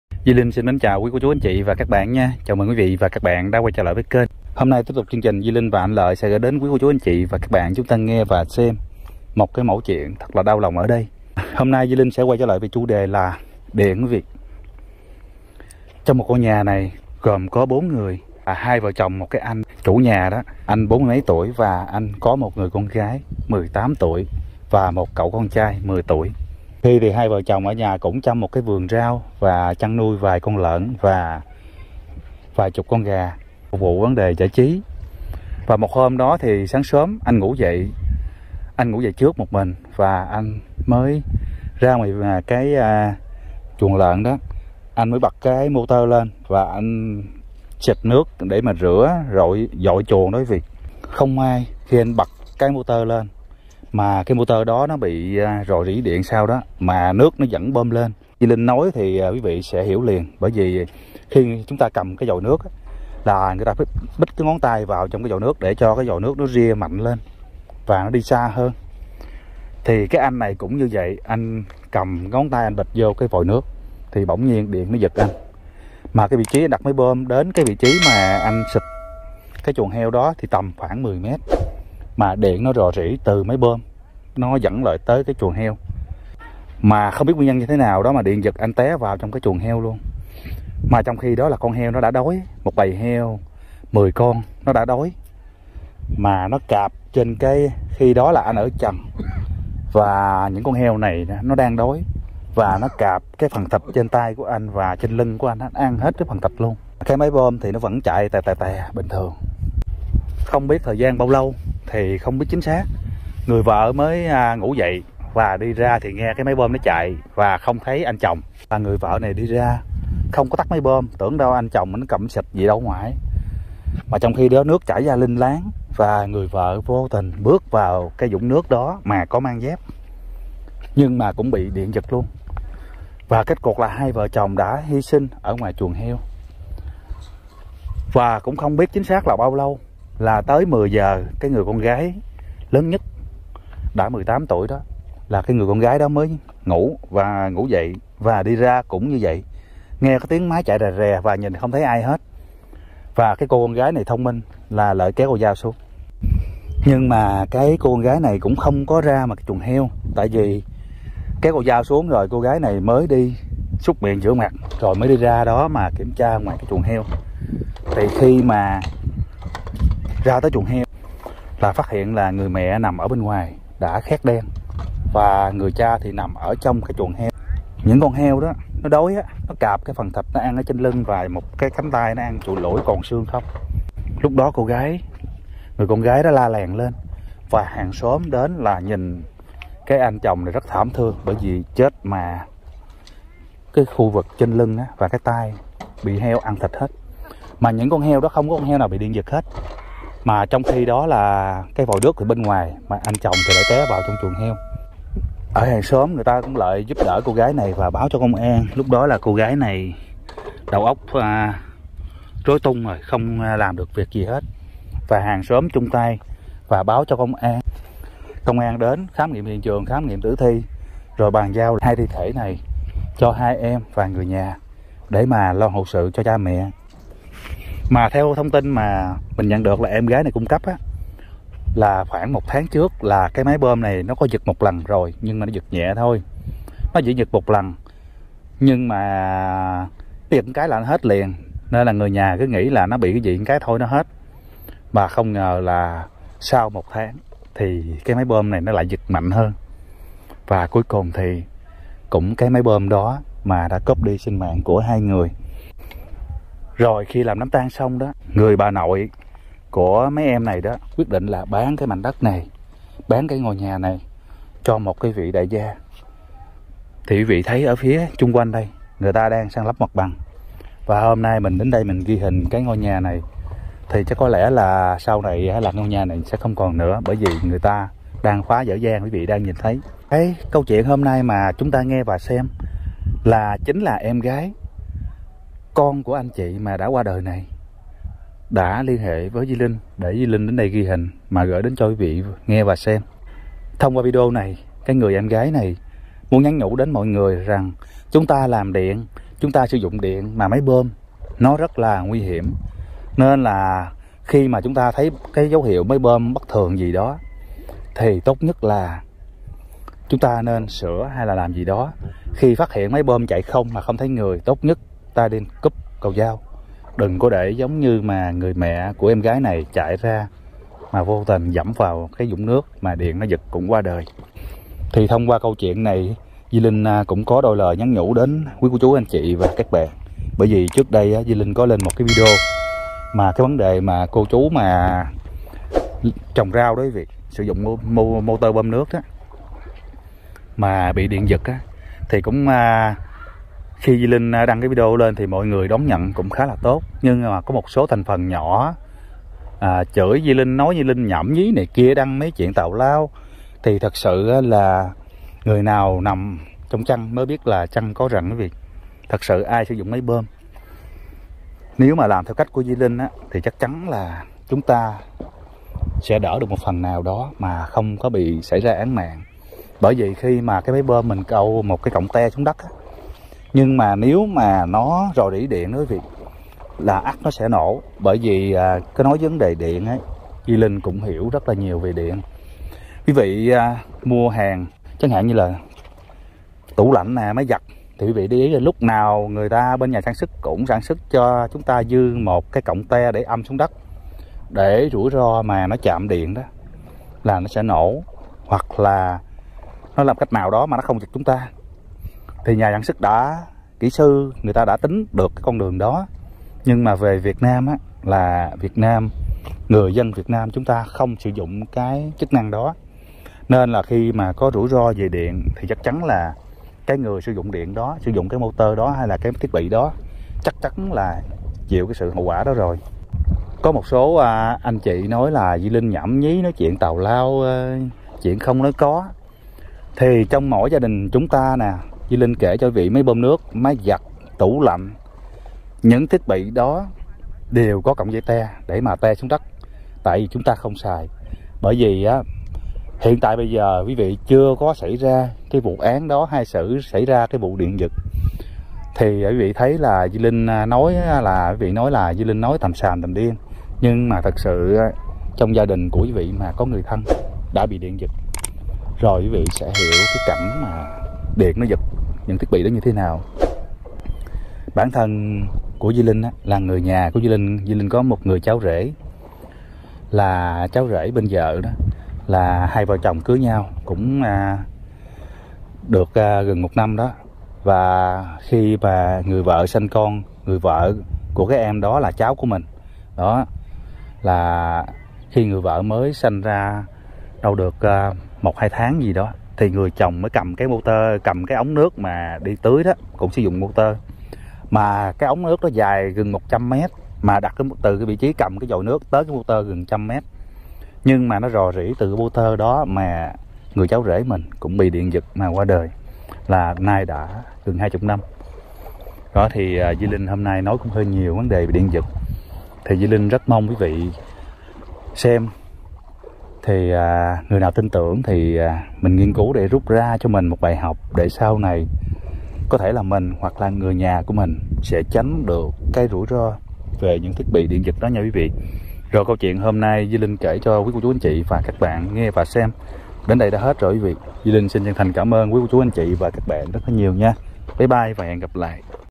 Kể truyện ma #57 | Hai vợ chồng chết thảm lúc tắm heo do điện giật từ máy bơm nước
Đầy là câu chuyện cảnh báo cho những người sử dụng máy bơm nước nhé